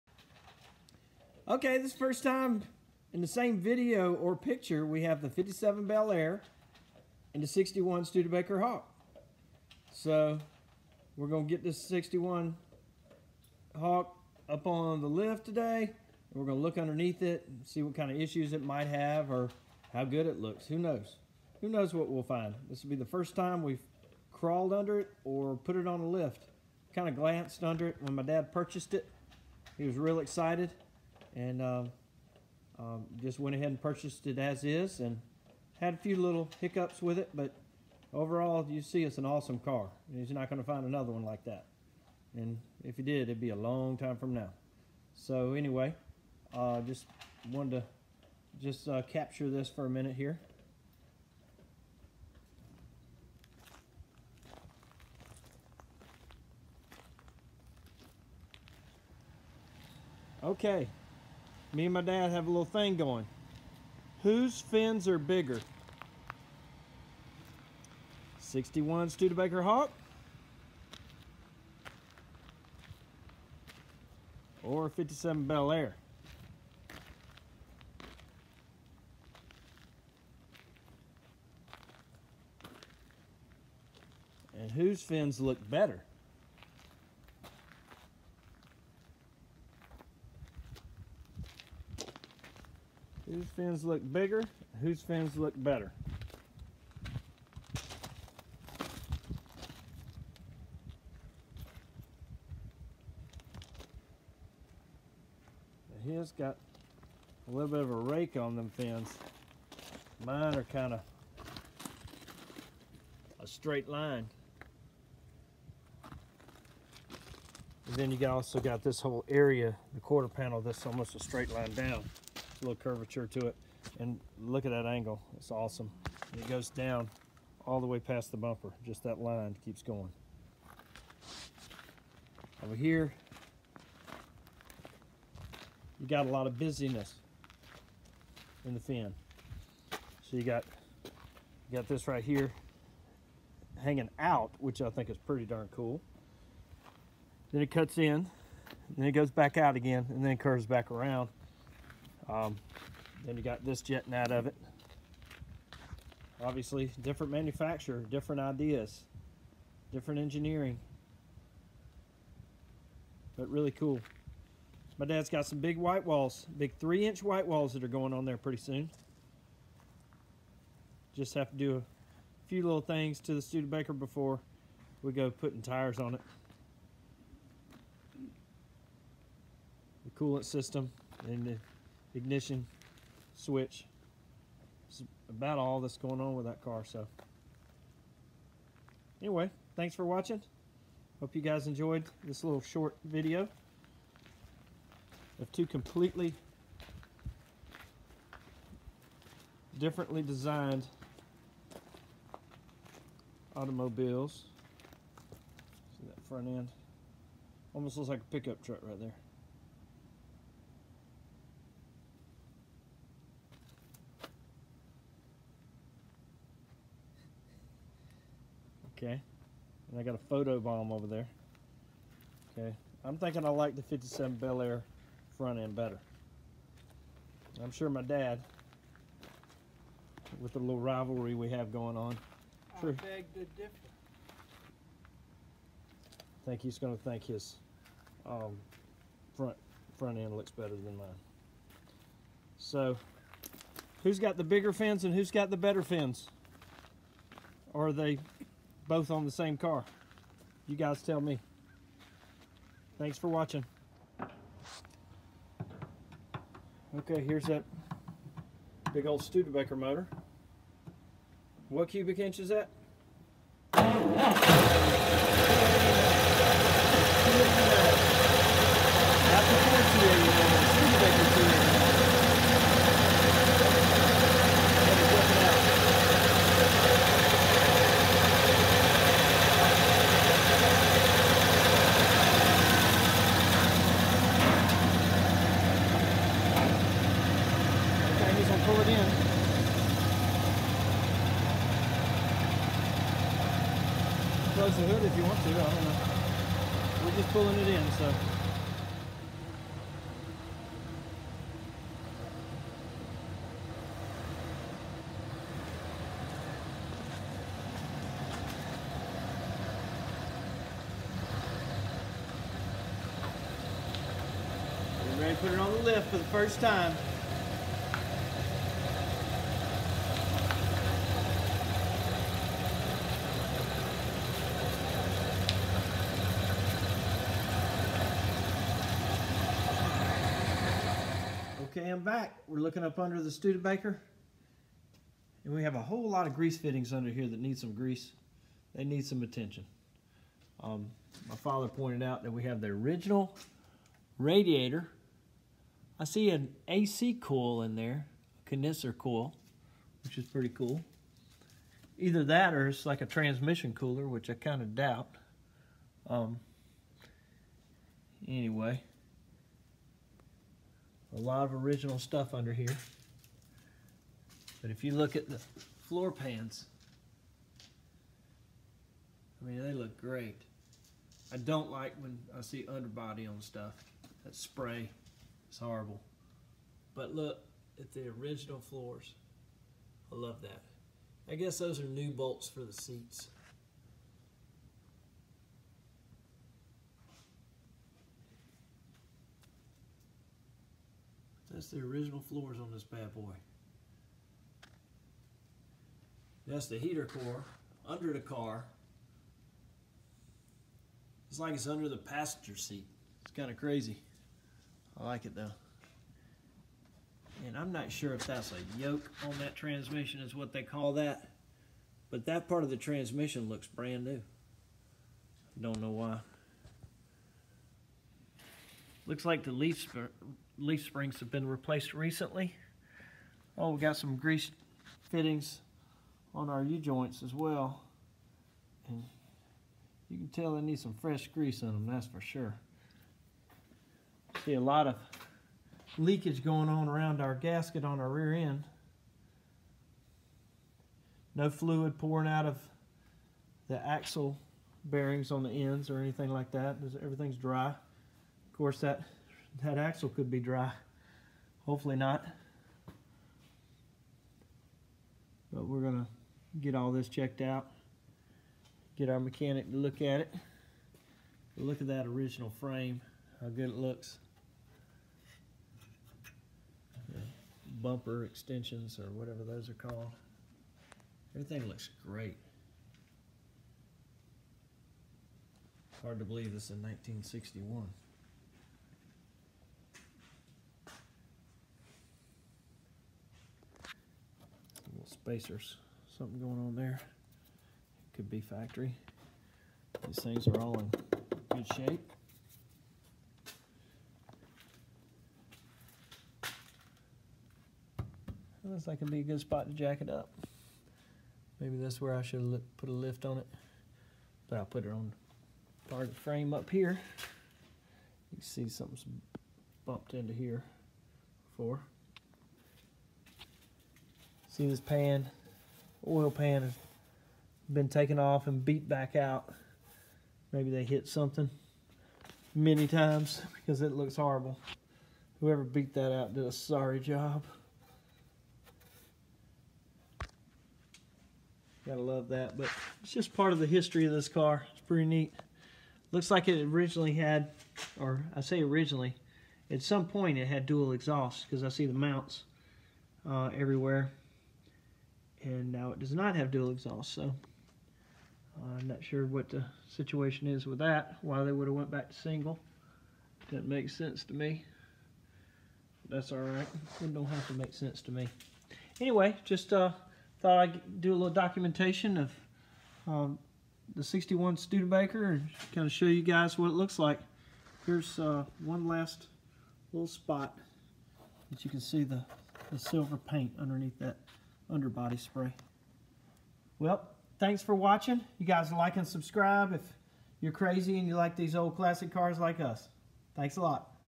Studebaker Hawk fins versus Chevrolet Belair fins and a look under the new Hawk
You be the judge. Whos fins are bigger and whos fins are better??? Also we got our first look under Dad's Hawk. South Carolina car 77000 miles and no rust! Original floor pans!!!